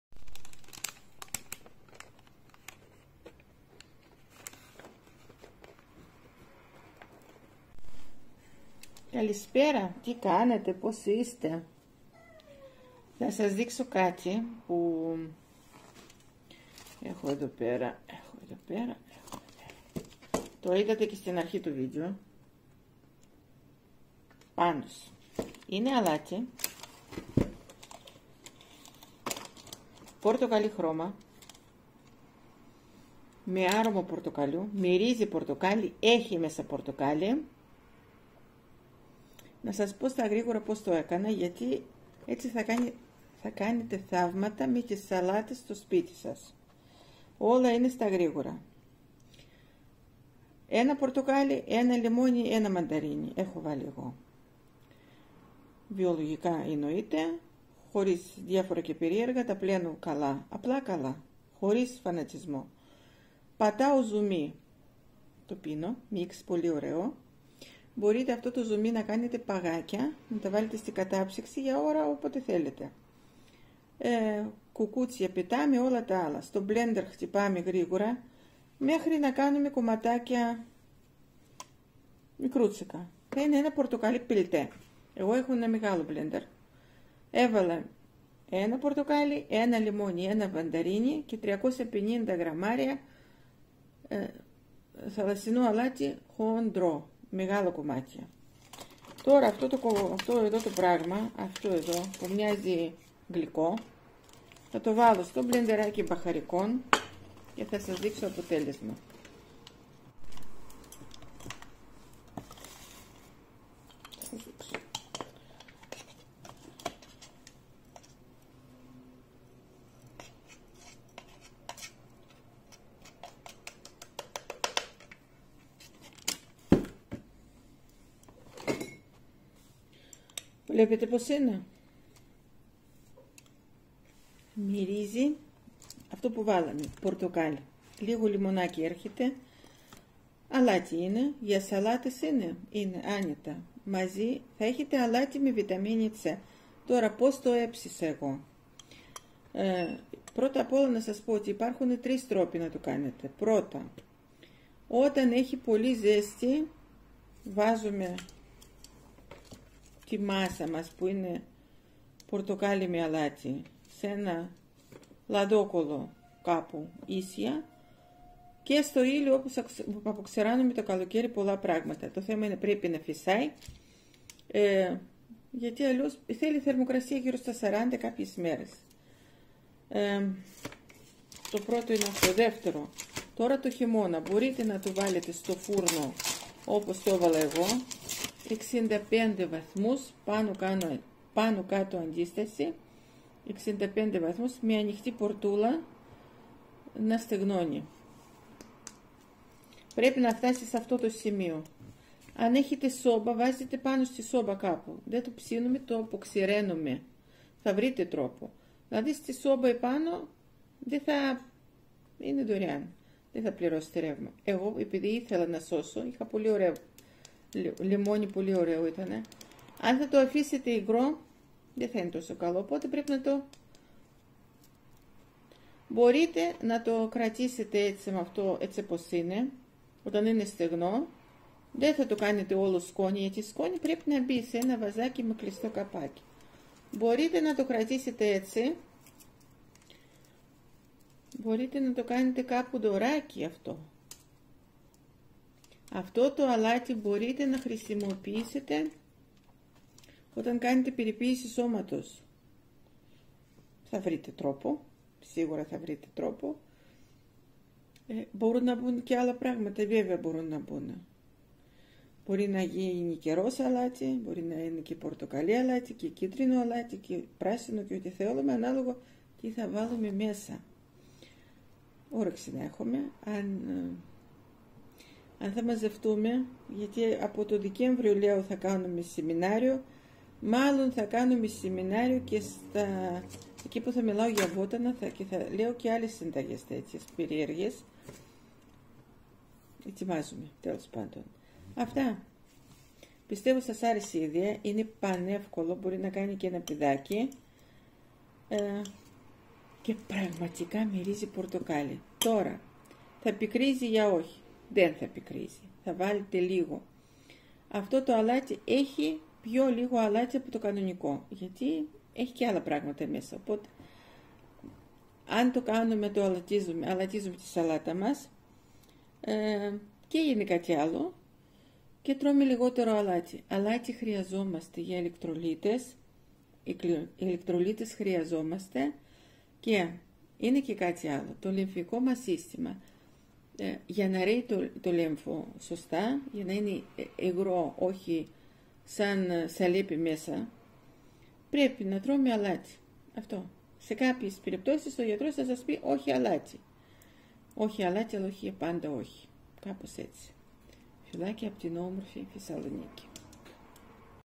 Αλάτι με Λεμόνι και Πορτοκάλι
Το φτιάχνω γιατί μου αρέσει τρελά το αποτέλεσμα. Το φτιάχνω γιατί θέλει πραγματικά μόνο 5 λεπτά από το χρόνο μου ( και πολλες ώρες μετά, αλλά εκεί δεν ζορίζομαι καθόλου)
Αυτή την φορά έχω πάρει 1 μέτριο πορτοκάλι, 1 μεγάλο λεμόνι, 1 μανταρίνι και 350 γραμμάρια χοντρό θαλασσινό αλάτι
Που θα με βρείτε:
Όλγα
messenger Giouzakova Olga
κατάστημα Exclusive Olia Θεσσαλονίκη Κορδελιό Ζερβιδη 8
ηλεκτρονικό κατάστημα